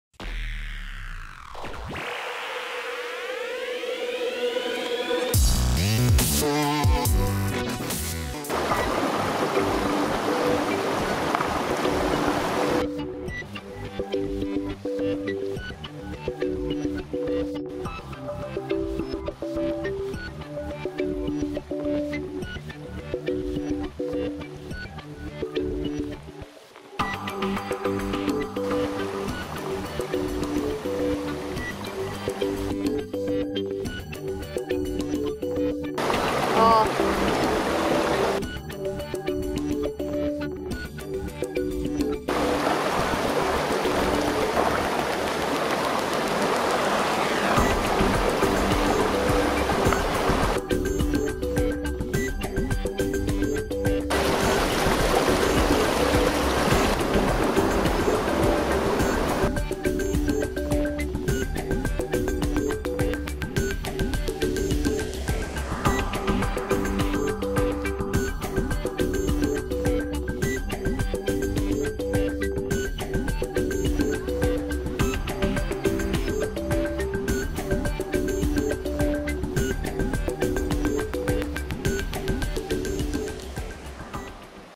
Impressions of Waldnaab l alpinsports tv
In diesem Video seht ihr viele Natureindrücke aus dem Waldnaabtal, z.B. Wasserläufe (auch in Zeitlupe) oder das Flussdelta mit dem bekannten "Butterfassl" oder eben auch Pflanzen aus dem Waldnaabtal.